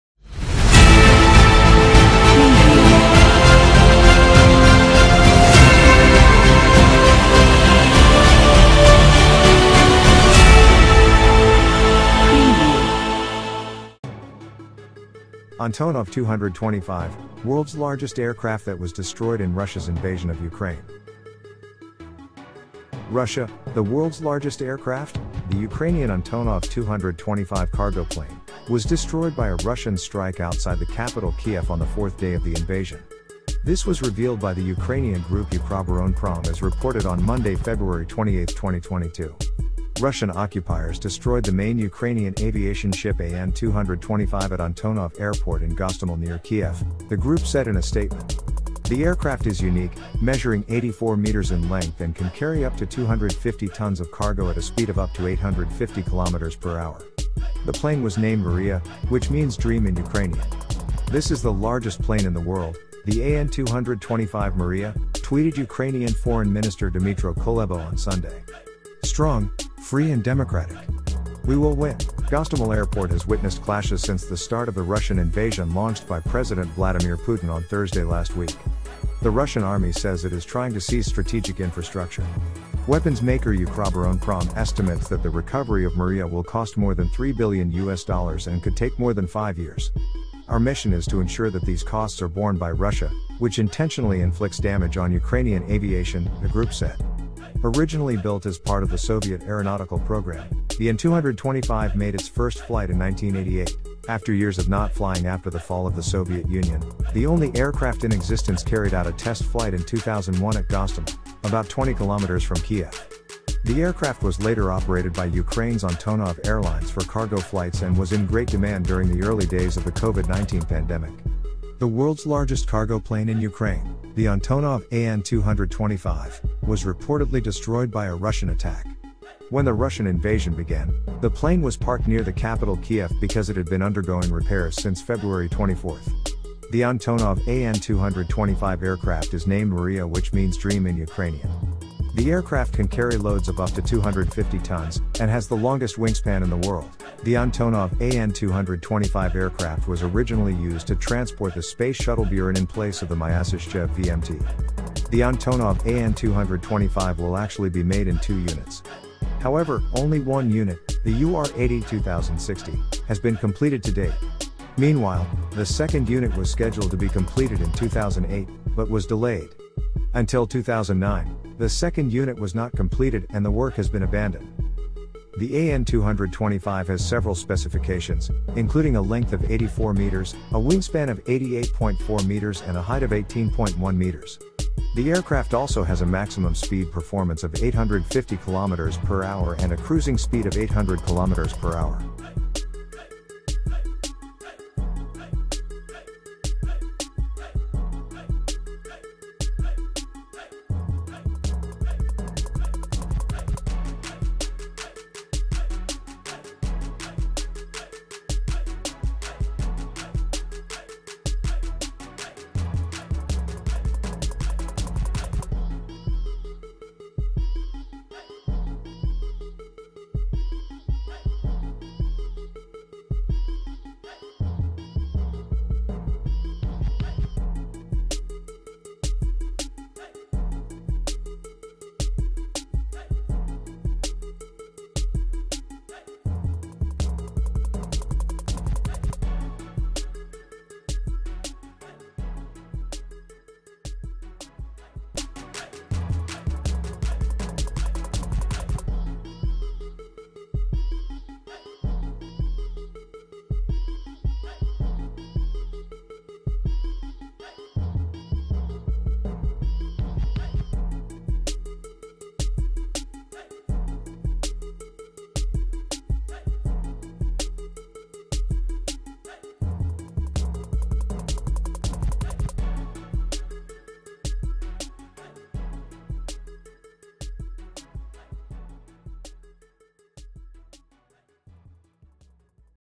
🟢 Antonov 225, World's Largest Aircraft Destroyed in Russia's Invasion of Ukraine
Antonov 225, World's Largest Aircraft That Was Destroyed in Russia's Invasion of Ukraine.
Russia - The world's largest aircraft - the Ukrainian Antonov-225 cargo plane - was destroyed by a Russian strike outside the capital Kyiv on the fourth day of the invasion.
This was revealed by the Ukrainian group Ukroboronprom as reported on Monday, February 28, 2022.
"Russian occupiers destroyed the main Ukrainian aviation ship AN-225 at Antonov airport in Gostomel near Kyiv," the group said in a statement.
The aircraft is unique, measuring 84 meters in length and can carry up to 250 tons of cargo at a speed of up to 850 kilometers per hour.
The plane was named "Mriya", which means "dream" in Ukrainian.
"This is the largest plane in the world, the AN-225 'Mriya'," tweeted Ukrainian Foreign Minister Dmytro Kuleba on Sunday. strong, free and democratic. We will win!"
Gostomel Airport has witnessed clashes since the start of the Russian invasion launched by President Vladimir Putin on Thursday last week.
The Russian army says it is trying to seize strategic infrastructure.
Weapons manufacturer Ukroboronprom estimates that the recovery of "Mriya" will cost more than US$3 billion or around Rp43 trillion and could take more than 5 years.
"Our mission is to ensure that these costs are borne by Russia, which intentionally inflicts damage on Ukrainian aviation," the group said.
Originally built as part of the Soviet aeronautical program, the An-225 made its first flight in 1988.
After years of not flying after the fall of the Soviet Union, the only aircraft in existence carried out a test flight in 2001 at Gostomel, about 20 kilometers from Kyiv.
The aircraft was later operated by Ukraine's Antonov Airlines for cargo flights and was in great demand during the early days of the COVID-19 pandemic.
The world's largest cargo plane in Ukraine, the Antonov AN-225, was reportedly destroyed by a Russian attack. When the Russian invasion began, the plane was parked near the capital Kiev because it had been undergoing repairs since February 24.
The Antonov AN-225 aircraft is named "Mriya" which means "dream" in Ukrainian. The aircraft can carry loads of up to 250 tons, and has the longest wingspan in the world.

The Antonov AN-225 aircraft was originally used to transport the space shuttle Buran in place of the Myasishchev VM-T.
The Antonov AN-225 will actually be made in two units. However, only one unit, the UR-82060, has been completed to date. Meanwhile, the second unit was scheduled to be completed in 2008, but was delayed. Until 2009, the second unit was not completed and the work has been abandoned.